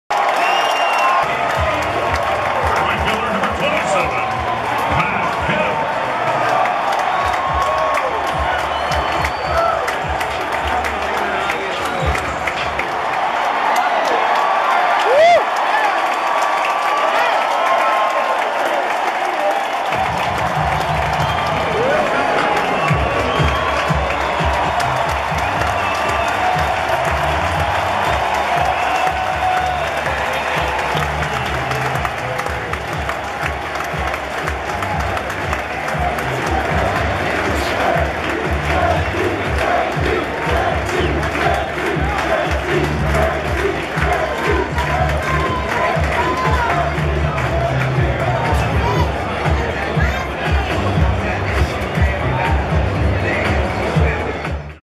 Zack Greinke is a One Man Gang in NLDS 10-4-2014
Greinke scores the 2nd run, dives head first into 3rd after going 1st to 3rd on a single, and leaves the game in the 8th inning winning 2-0. The Dodgers channel is replaying this game right now (2/19/2015).